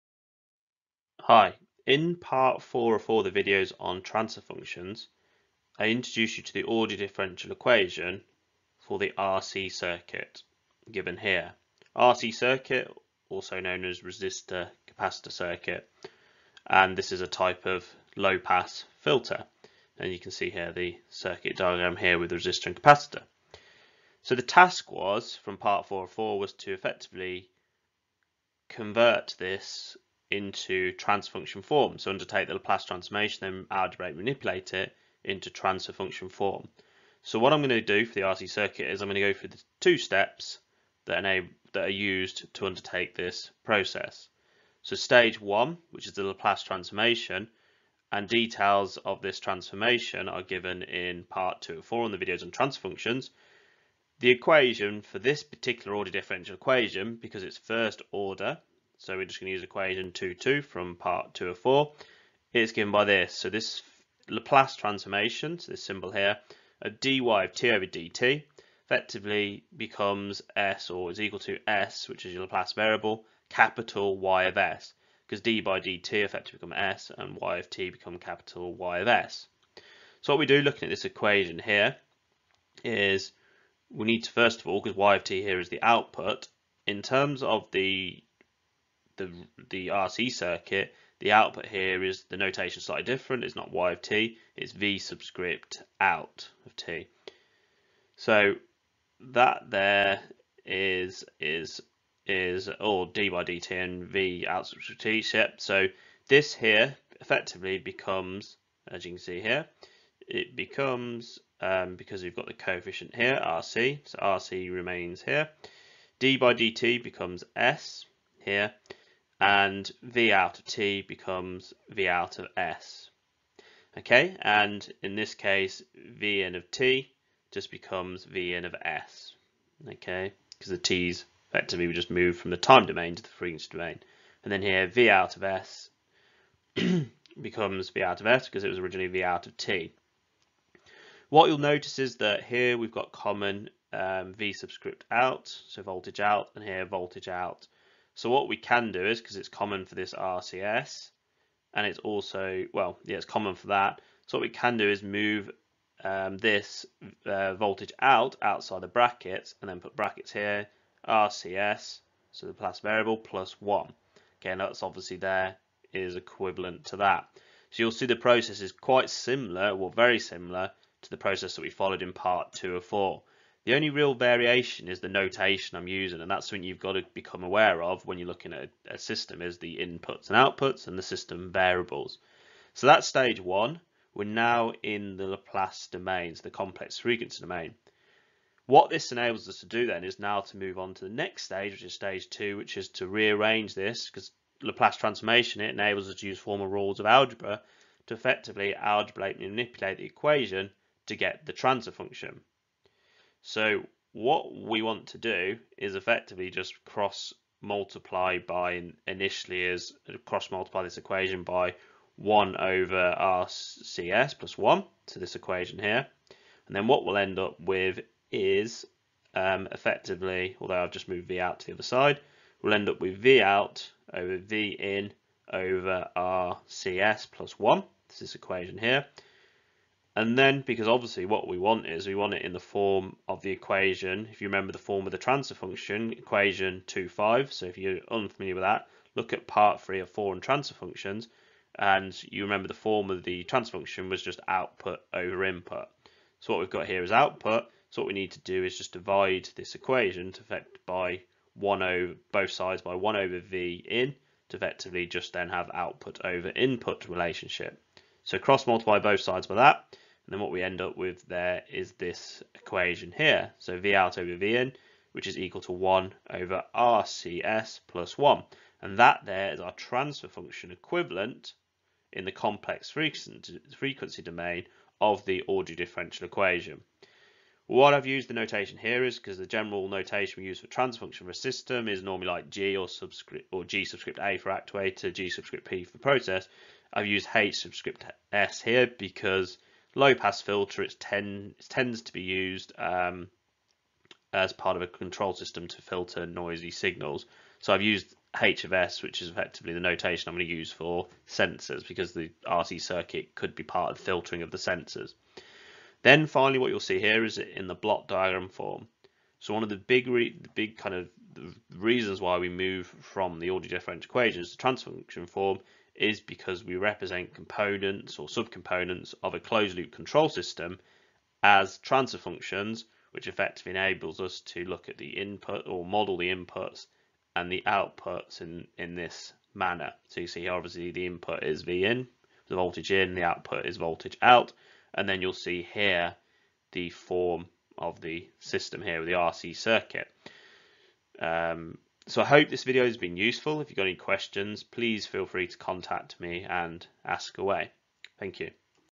Transfer Functions: RC Circuit (Applied Example)
The video details how to convert a resistor-capacitor (RC) ordinary differential equation (ODE) into a transfer function, via the Laplace transformation.